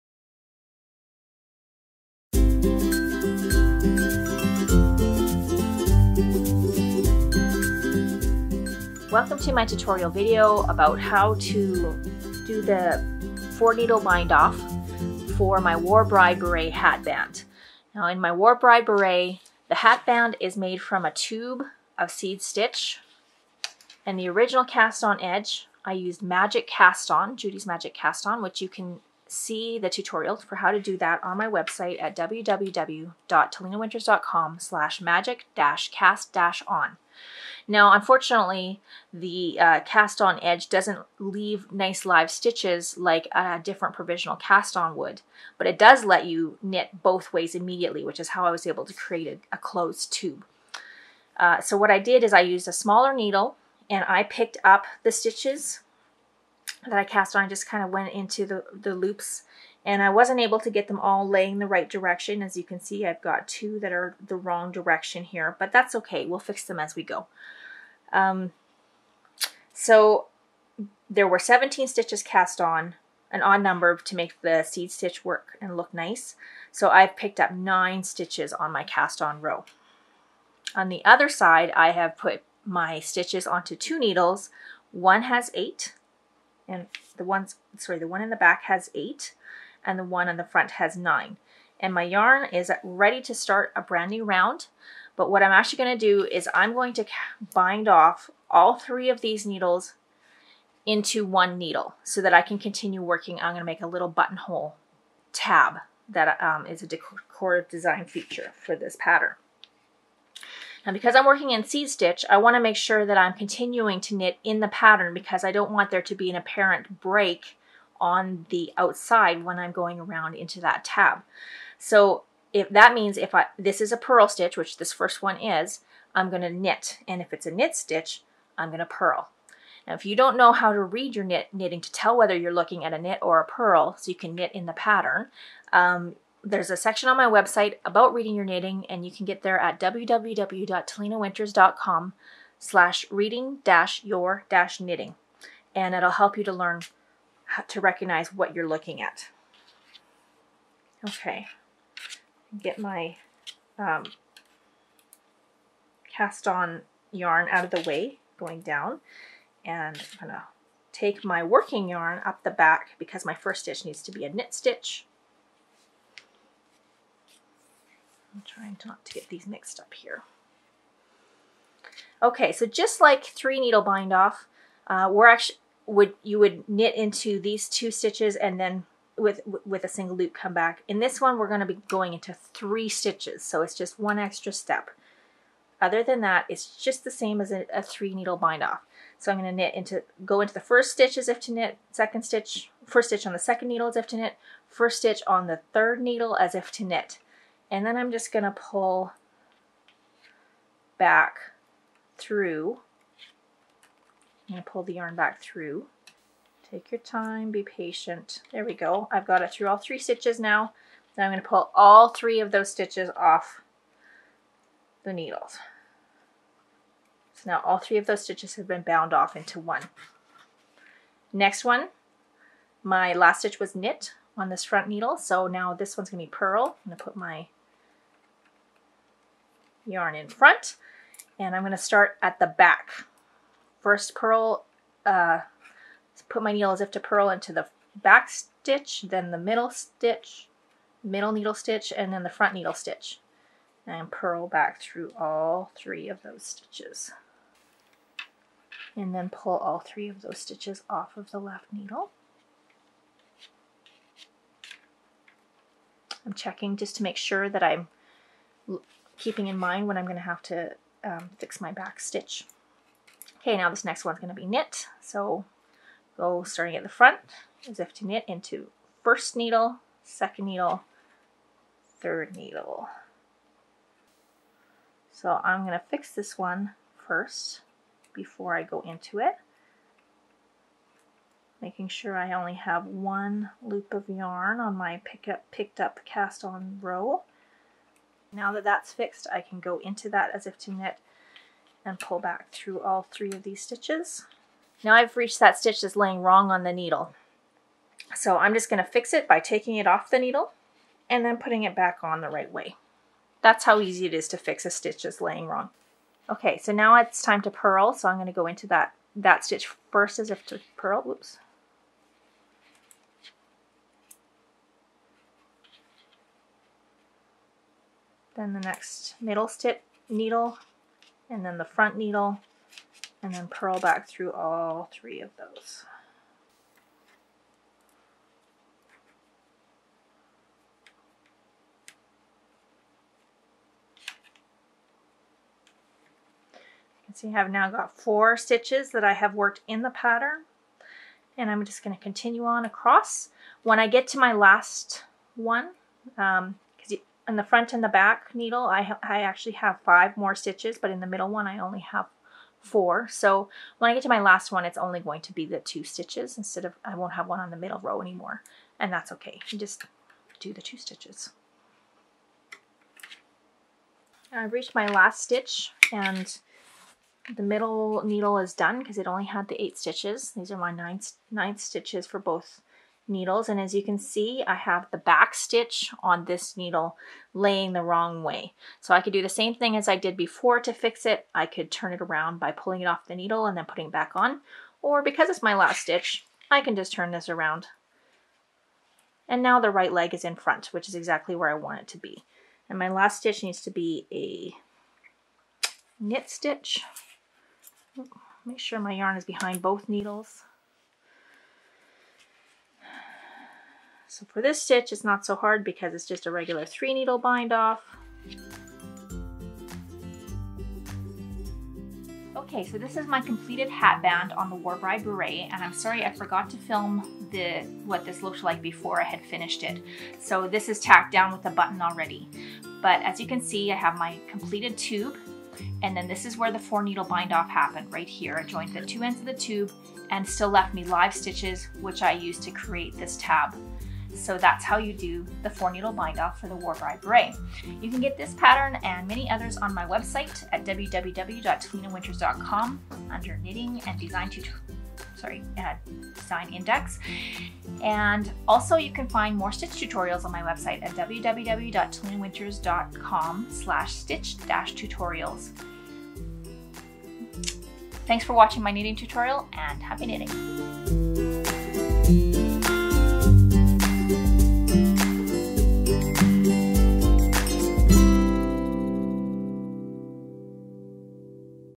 4-Needle Knit Bind Off Technique (Join 3 sets of live stitches)
This bind off shows how to join two ends of a tube that is closed on one end and open on the other, so I join three sets of live stitches into one.

This is similar to 3-Needle Bind Off, only with an extra needle. Find my video and photo tutorial for 3-Needle Bind Off

How to Read Your Knitting series (to read your knit and purl stitches) is

Happy knitting!

Talena

This video originally appeared on my author website and channel Any URLs mentioned in the video will redirect to my designer